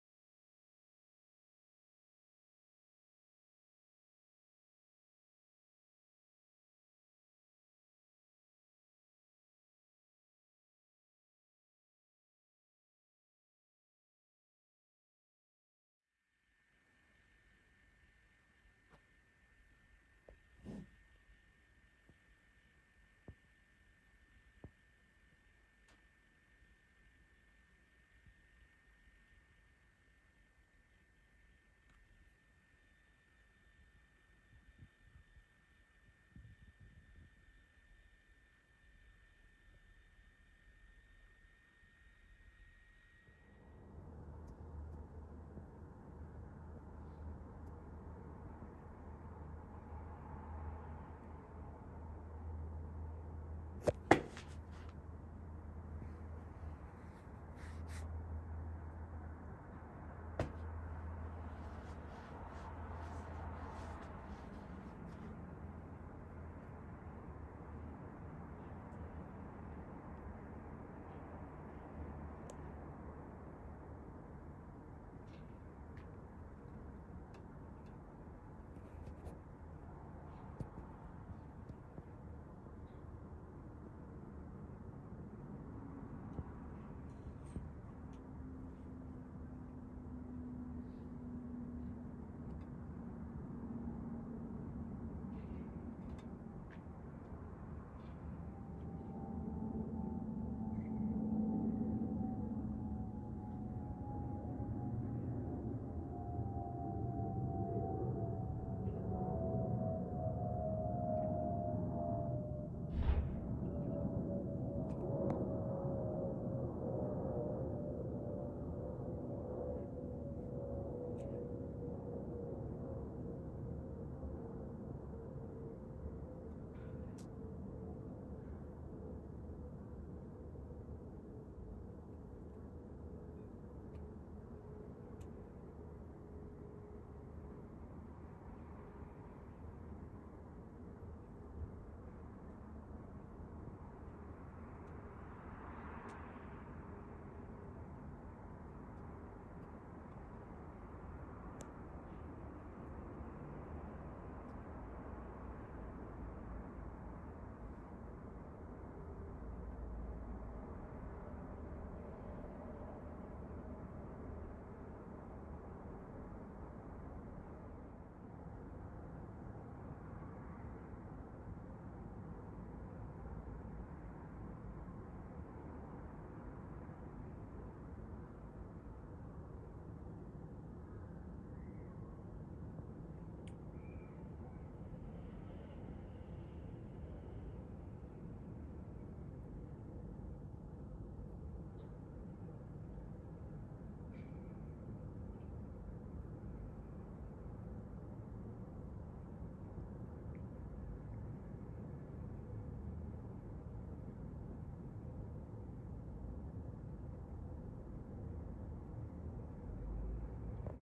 FOPP Approved Brussels Airlines Airbus A320 [OO-SNK] overhead night flyover
Here is another Airbus 320 specimen retaining the sacred FOPP howl that I have managed to capture in the void.

Apologies for the epileptic camera movement as I had to eradicate a creepy crawly webbing around my left limb at the time of recording